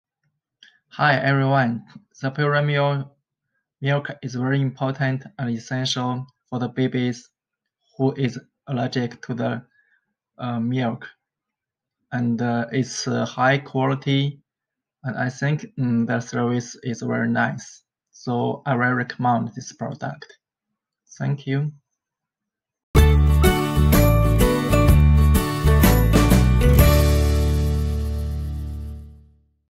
PURAMINO A+ review by GUIBIN
*Received discount
PURAMINO A+ is an amino acid-based formula scientifically designed for effective dietary management of infants & toddlers with severe cow's milk protein & multiple food allergies or other conditions requiring an amino acid-based formula.